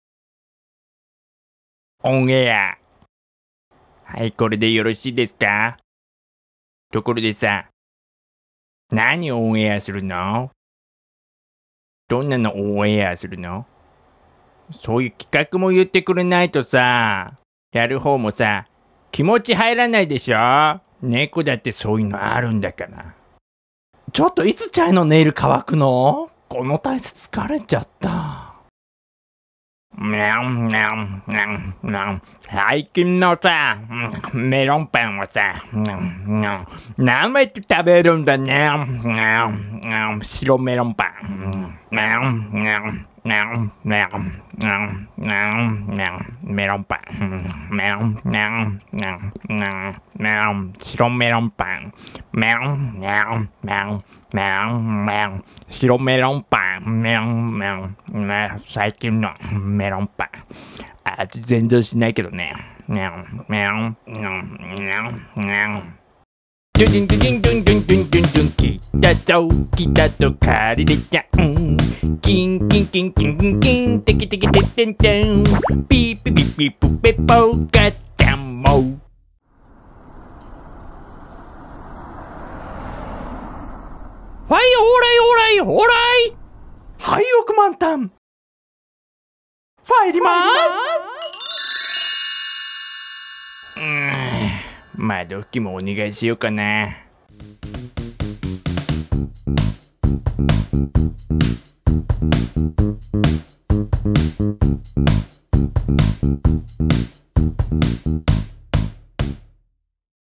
もしも猫がしゃべったら「YouTuber猫の気持ち」「メロンパンを食べる猫」ほか
かりんちゃん（茶トラ白猫）ちゃいちゃん（三毛猫）ショートコントシリーズ

moshinukoとは...